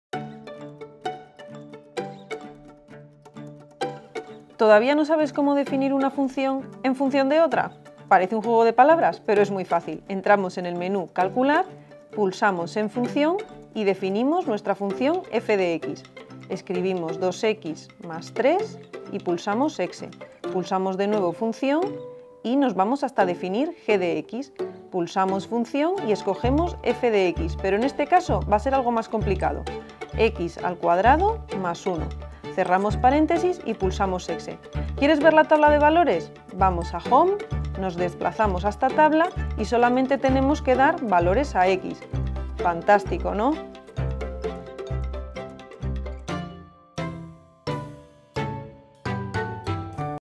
Casio ClassWiz - Función compuesta
Si todavía no has aprendido a definir una función en función de otra con tu calculadora, en este tutorial te enseñamos cómo puedes hacerlo.